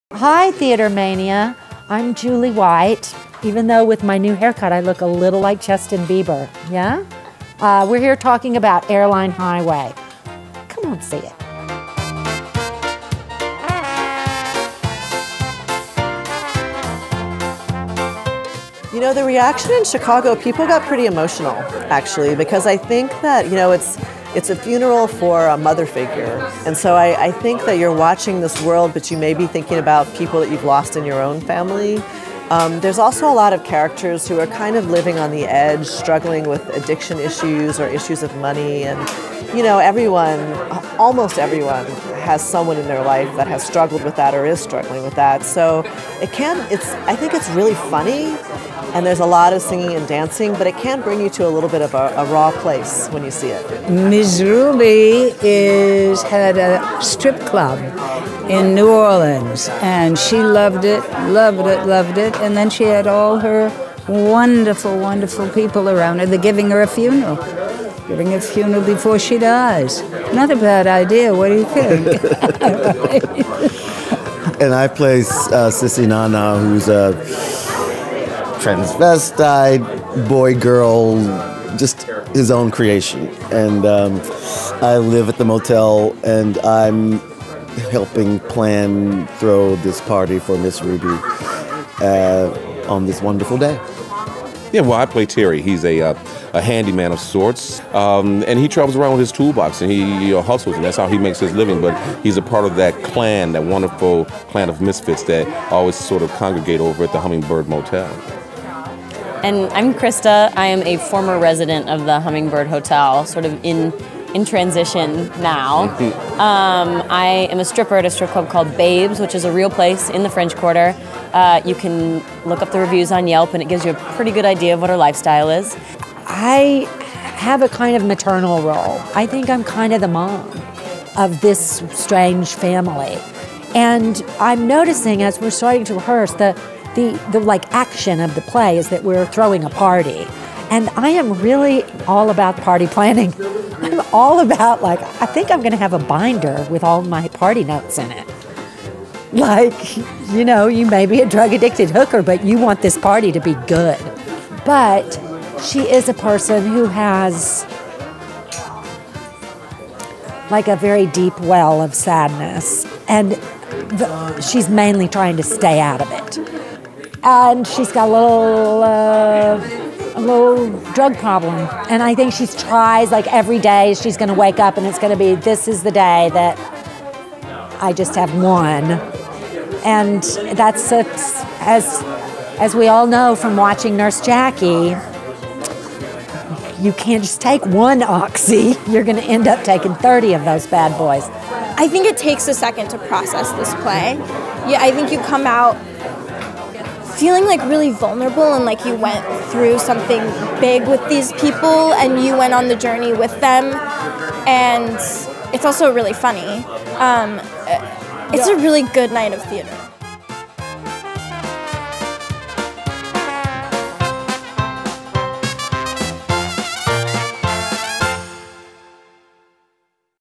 Julie White and the Cast of Broadway's Airline Highway Introduce Lisa D'Amour's Eccentric Characters
The sizeable cast of Lisa D'Amour's Airline Highway recently gathered to meet the press in advance of the play's Broadway premiere at the Samuel J. Friedman Theatre on April 23. The energetic crew, including Tony winner Julie White and more, spoke with TheaterMania about the characters they portray and the moving nature of this bold new piece of theater.